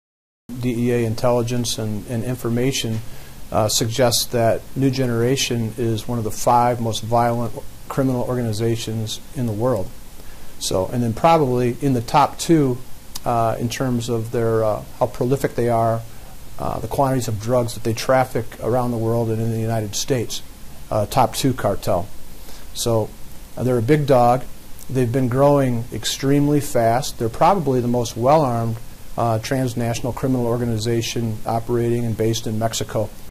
CBS 2 Exclusive On 'Project Python'
Robert J. Bell, Special Agent in Charge, Chicago Field Division talks about the major dent in the operations of a notorious drug cartel. A nationwide sting netted several hundred people.